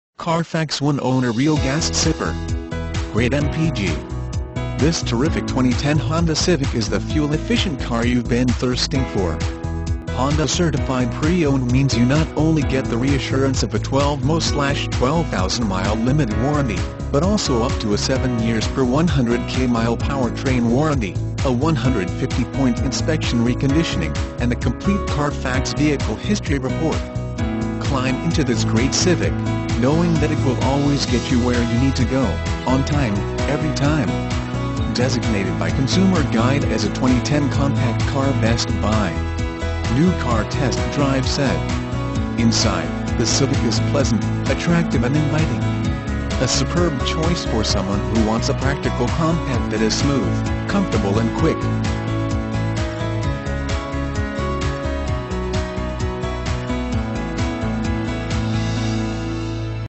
2010 HONDA CIVIC COUPE Webster, TX AH501589
2010 HONDA CIVIC COUPE Webster, TX
Stock# AH501589

Honda of Clear Lake (HondaofClearLakeUSED)
20233 Gulf Fwy

CARFAX ONE OWNER. Real gas sipper! Great MPG! This terrific 2010 Honda Civic is the fuel-efficient car you've been thirsting for. Honda Certified Pre-Owned means you not only get the reassurance of a 12mo/12,000 mile limited warranty, but also up to a 7yr/100k mile powertrain warranty, a 150-point inspection/reconditioning, and a complete CARFAX vehicle history report. Climb into this great Civic, knowing that it will always get you where you need to go, on time, every time. Designated by Consumer Guide as a 2010 Compact Car Best Buy. New Car Test Drive said  ...Inside, the Civic is pleasant, attractive and inviting...a superb choice for someone who wants a practical compact that is smooth, comfortable and quick...